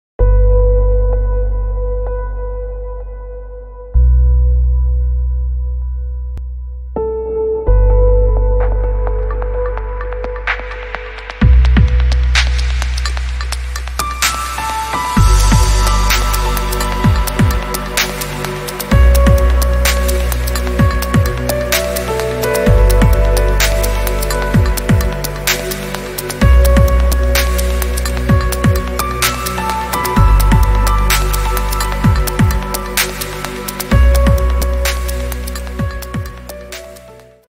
HC-20 Double ended terminal crimping machine
Simple operation with touch screen
Ultra-quick changeover, setup and cycle time
Unique quality monitoring devices